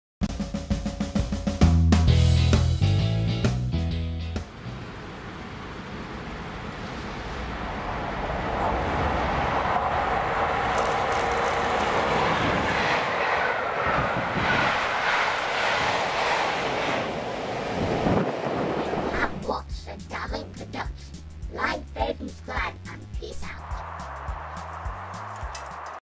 #905: [ECS] WCRC Class 47237 & 57315 pass Warrington Bank Quay (03/06/17)
West Coast Class 47237 & 57315 pass Warrington Bank Quay with an ECS (5Z63) from Carnforth Steamtown to London Victoria